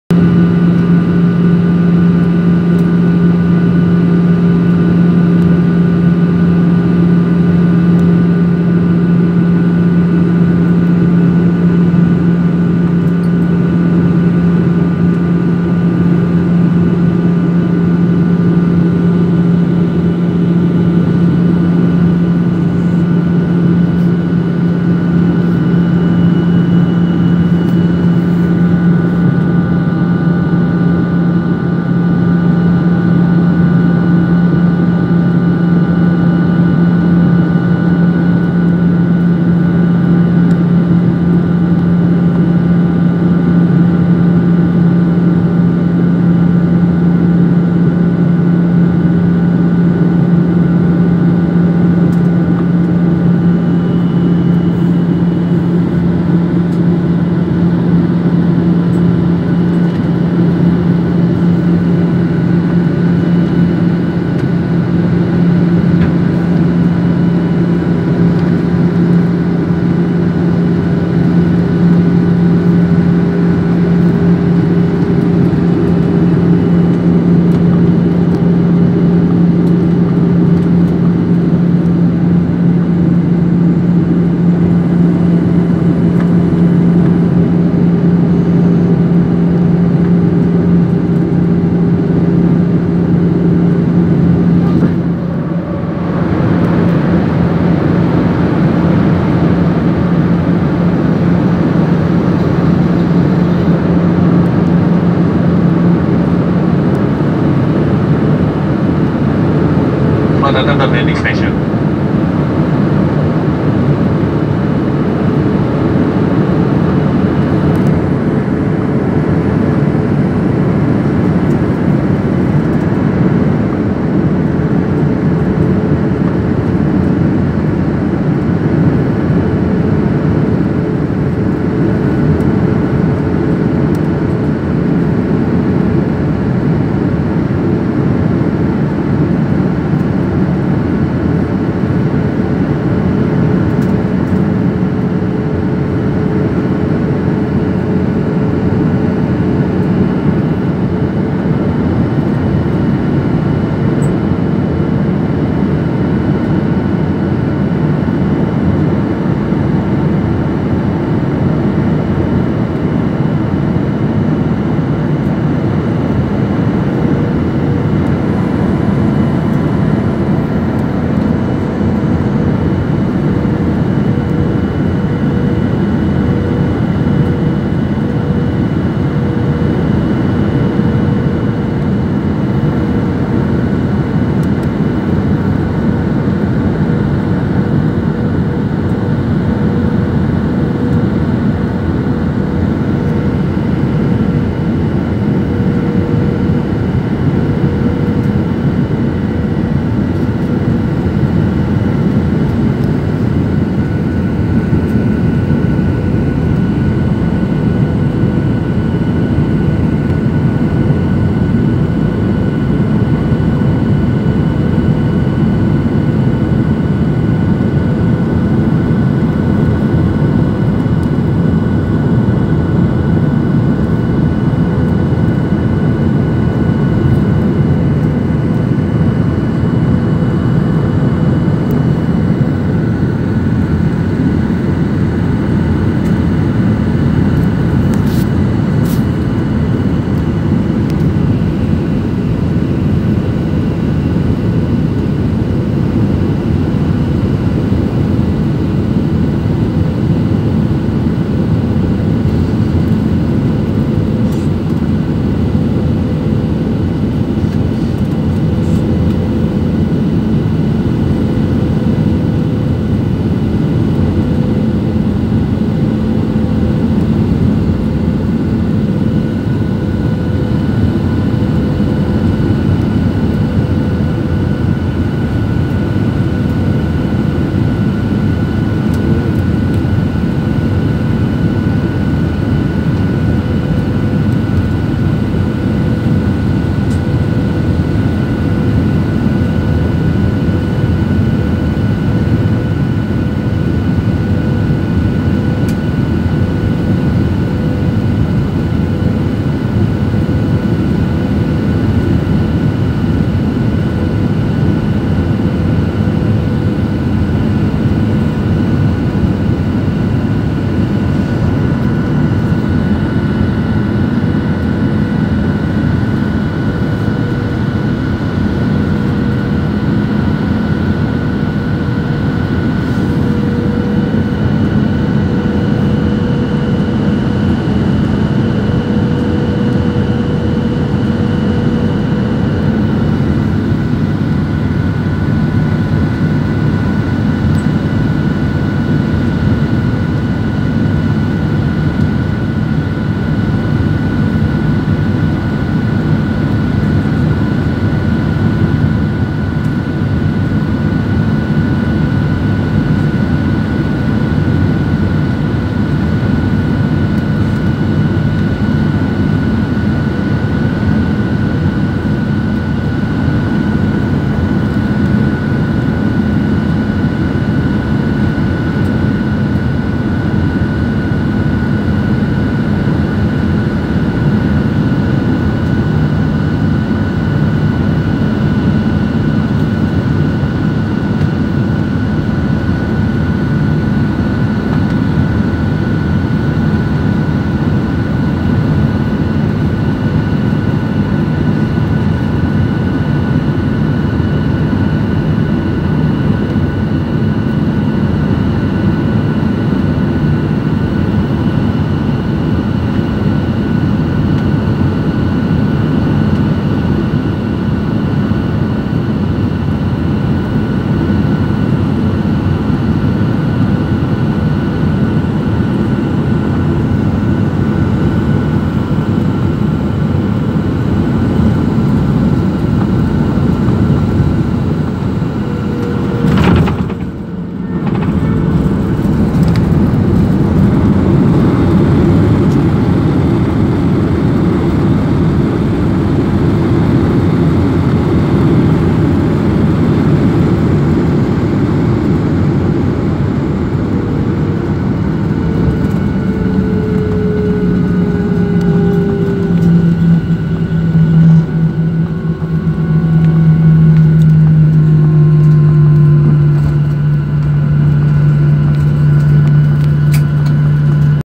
Setelah berputar di atas laut...Mendarat di bandara baru New Yogyakarta International Airport (YIA)
Pesawat bandara plane airport